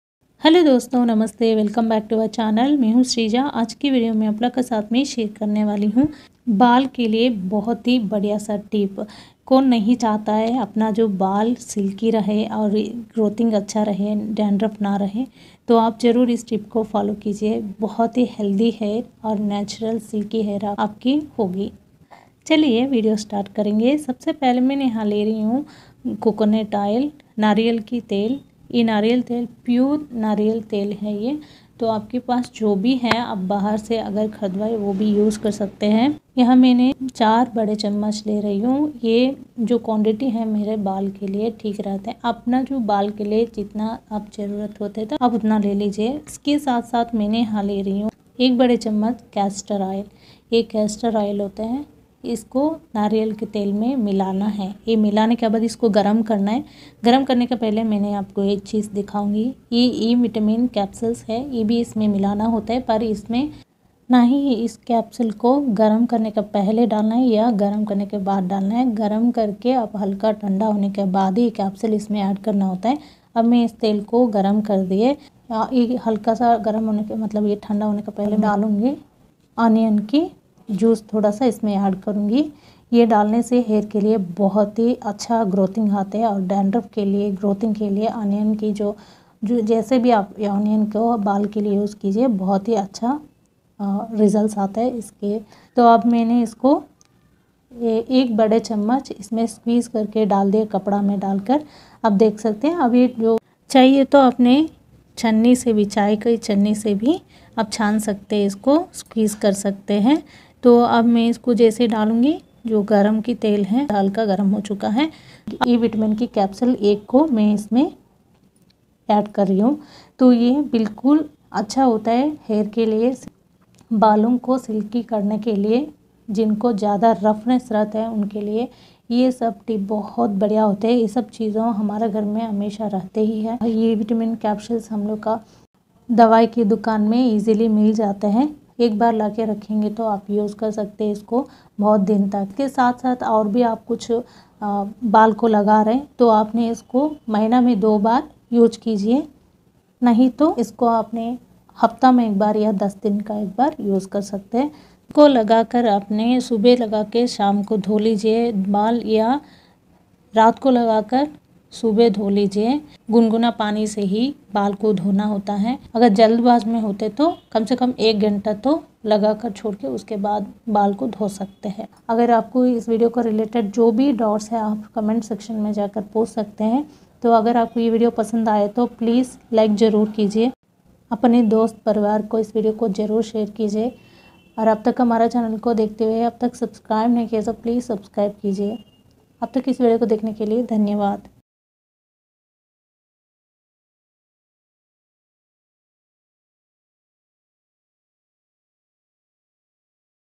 1ही vash me बाल smooth -silky -shiny हो जाएंगे| Hair care DIY| बिल्कुल घर में रहने चीजों से ही
1ही vash me बाल smooth -silky -shiny हो जाएंगे| Hair care DIY| बिल्कुल घर में रहने चीजों से ही